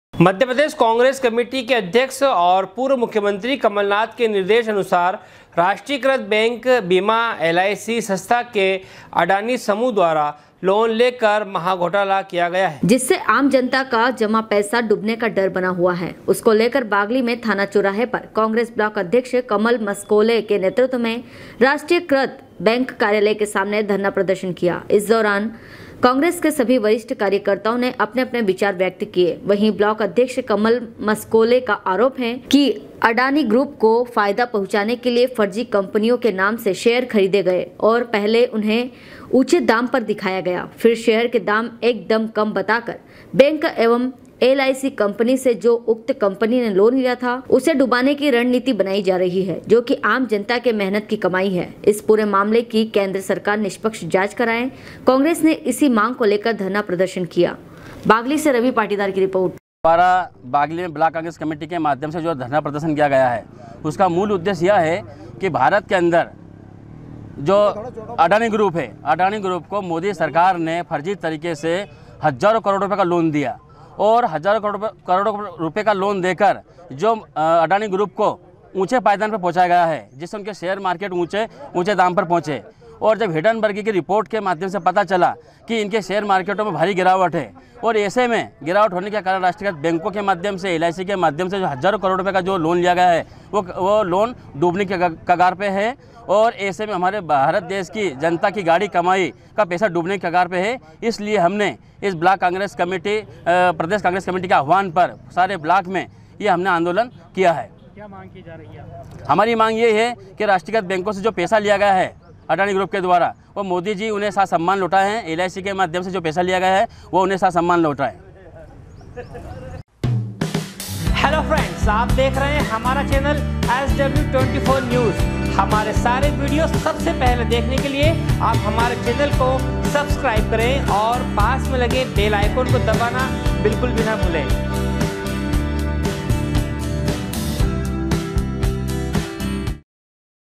मध्य प्रदेश कांग्रेस कमेटी के अध्यक्ष एवं पूर्व मुख्यमंत्री कमलनाथ के निर्देश अनुसार
मध्य प्रदेश कांग्रेस कमेटी के अध्यक्ष एवं पूर्व मुख्यमंत्री कमलनाथ के निर्देश अनुसार राष्ट्रीय कृत बैंक बीमा एलआईसी संस्था से अडाणी समुह द्वारा लोन लेकर महा घोटाला किया गया।